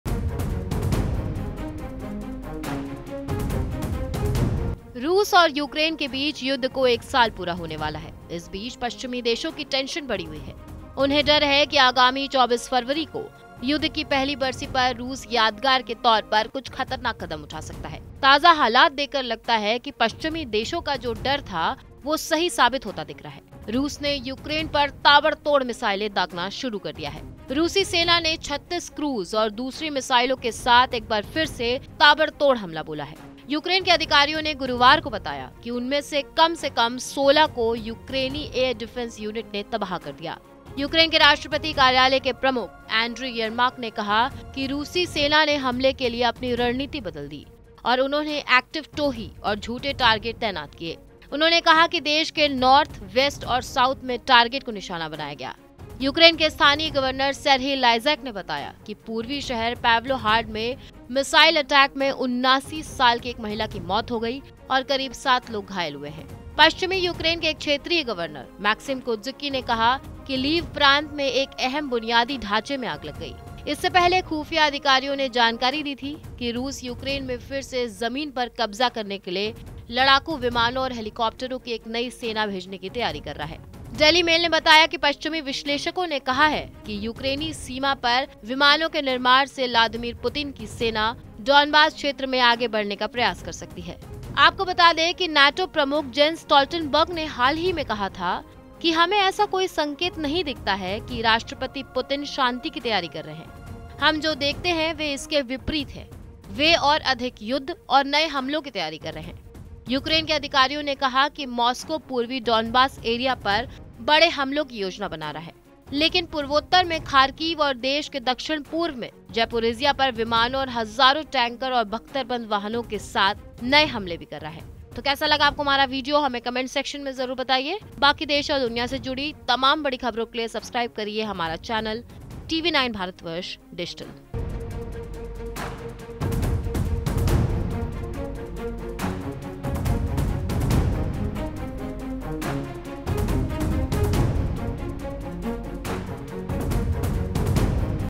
Russia Ukraine War: रूस ने यूक्रेन पर दागी ताबड़तोड़ मिसाइलें ! | Missile Attack | Putin
यूक्रेन के साथ जारी युद्ध में रूस की सेना ने 36 क्रूज और अन्य मिसाइलों के साथ एक बार फिर से ताबड़तोड़ हमला बोला है। यूक्रेन के अधिकारियों ने गुरुवार को बताया कि उनमें से कम से कम 16 को यूक्रेनी वायु रक्षा इकाइयों ने नष्ट कर दिया। यूक्रेन के राष्ट्रपति कार्यालय के प्रमुख ने कहा कि देश के उत्तर, पश्चिम और दक्षिण में लक्ष्यों को निशाना बनाया गया। यूक्रेन के स्थानीय गवर्नर सेरही लिसाक ने बताया कि पूर्वी शहर पावलोह्राद में मिसाइलों के हमले में 79 वर्षीय एक महिला की मौत हो गई और कम से कम सात अन्य लोग घायल हो गए।